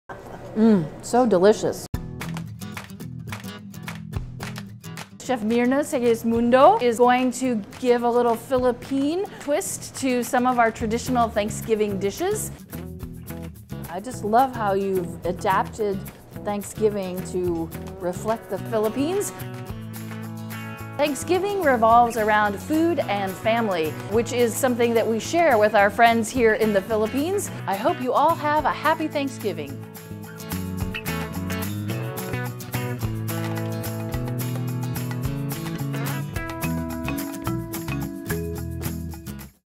Thanksgiving Spread with a Filipino Twist!
From all of us at the U.S. Embassy in the Philippines, we wish you a Happy Thanksgiving! Take a look at the American-Filipino fusion dishes put together by award-winning chef Myrna Segismundo.